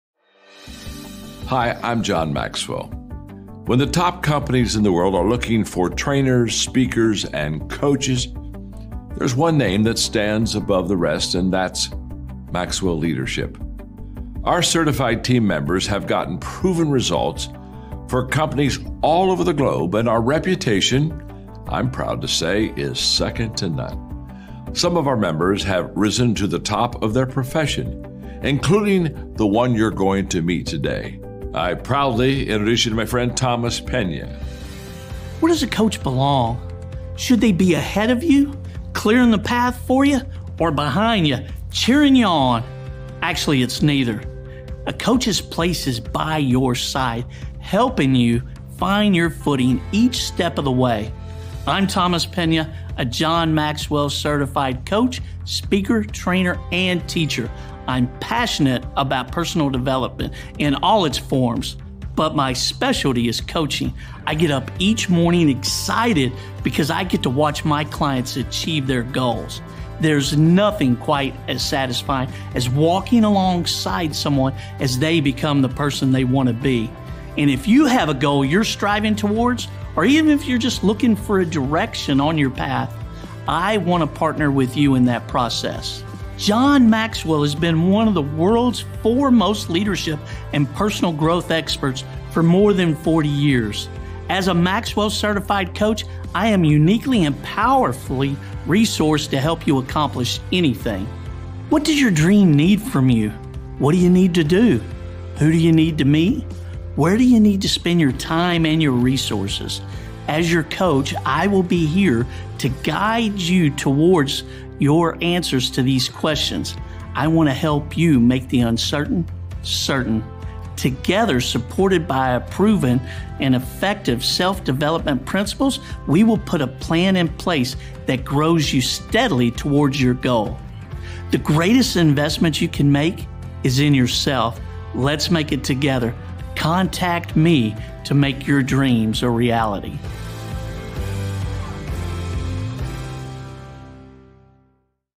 John Maxwell Intro
I don't know about you but, I always thought getting a coach was just for the rich and pro athletes. Little did I know that after I got a coach how my thoughts, actions, and how business and personal life changed. It was a great decision I made. I was so surprised that when I started looking and interviewing coaches so many of them had NO Credentials, NO SCHOOLING, NADA ZIP NOTHING!!! When I finally found the one coach I wanted, he was John Maxwell certified, now had read a few of John Maxwell's books, but I was so impressed, and the success I was having had me want to become a coach too. That's when I followed in my coach's footsteps and became a certified John Maxwell Coach, Trainer, and Speaker. So when picking your coach or training make sure they have the credentials to back them. I would love to help you in any way I can.